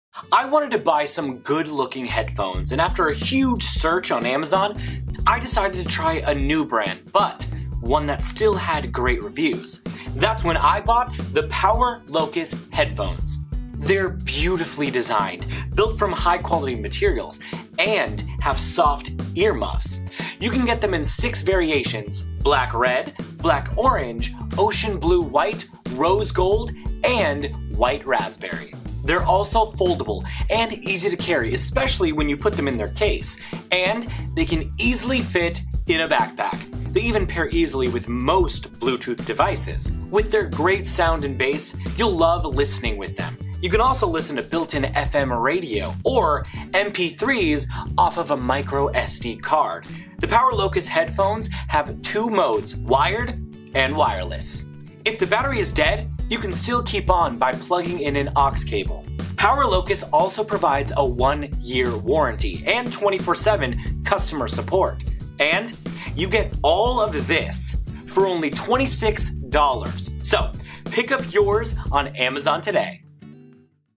One of the best wireless headphones for less than 30$
Here is our proposal for great quality wireless bluetooth headphones over ear.

- Concert Quality Audio
- Foldable with carry case incuded
- Extendable sides
- Micro SD/TF , Radio FM , Wired mode option
- Soft earmuffs for long use
- 1 year Full warranty